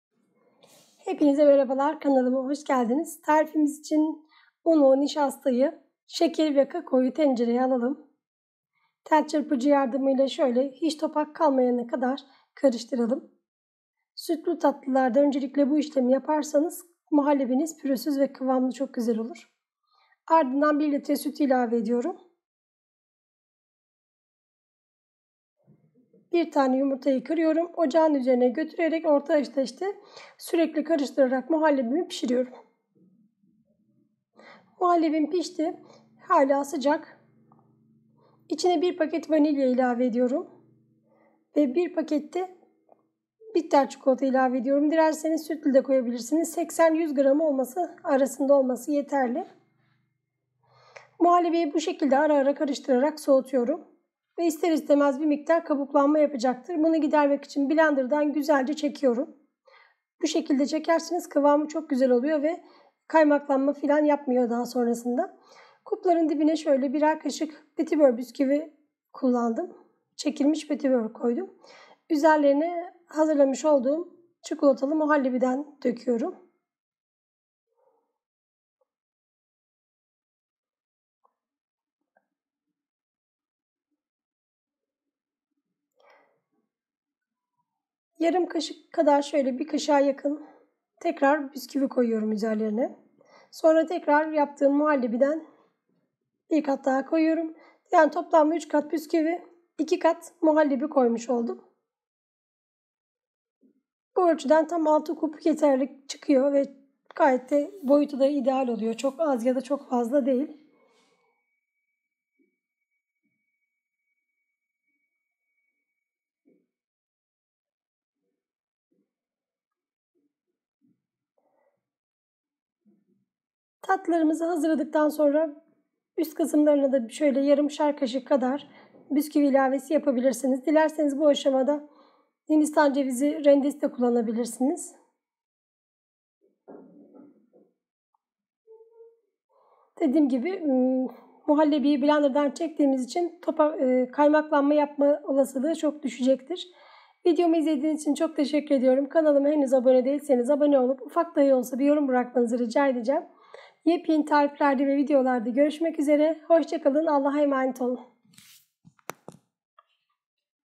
LEZZETİ İLE DAMAKLARI ŞENLENDİRECEK KUPTA ÇİKOLATALI MAGNOLİA
1 lt süt
2 şer yemek kaşığı nişasta ve un (tepeleme olmasın silme)
1 su bardağı şeker
3 yemek kaşığı kakao
1 paket vanilya
1 yumurta
1 paket çikolata
(Dilerseniz bir kutu krema da ekleyebilirsiniz)
Yarım paket petibör bisküvi
Bu tariften normal boy 6 kup tatlı çıkıyor dilerseniz ufak kuplarda 8 kup yapabilirsiniz. Şimdiden yapacak olanların eline sağlık. Tarifimi denerseniz yorum yazmayı unutmazsanız sevinirim.